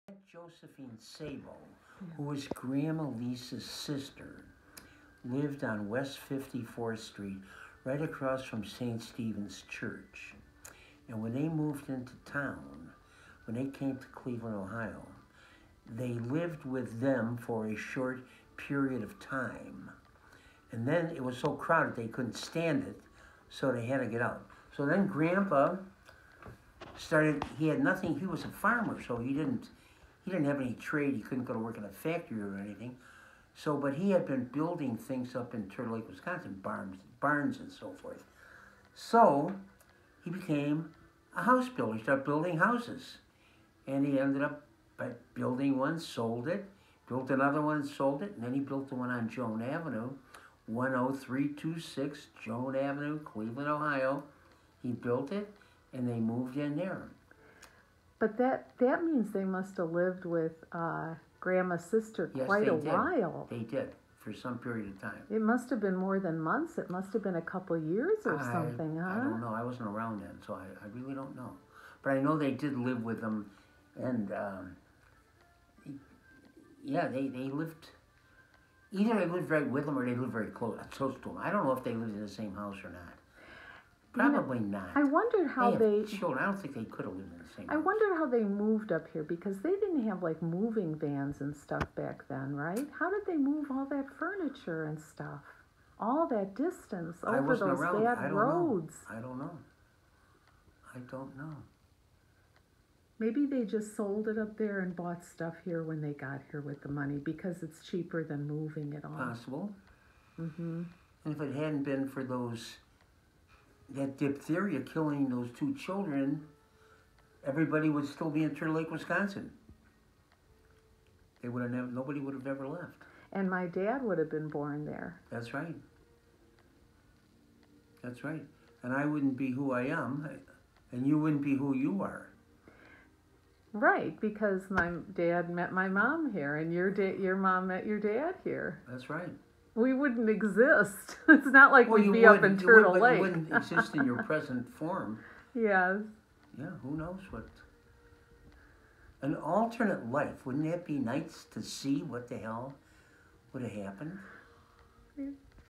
Joseph Leisz moves his family to Cleveland in 1919
In this short video, Billy Waters (b. 1931), eldest grandchild of Leisz Brother Joseph (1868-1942), tells how his grandpa moved his family from Turtle Lake, Wisconsin in 1919 to Cleveland, Ohio. Upon their arrival, the Leisz's first lived with Josephine Haspray Sabo, sister of Joseph's wife Louisa Haspray Leisz, and then found a place of their own for Joseph, Louisa and their six children. Their seventh child, Jack, would be the only one of their children born in Cleveland; and the couple left two children behind in Turtle Lake, Tracie, 9, and Franky, 4, who died from diphtheria in February 1918.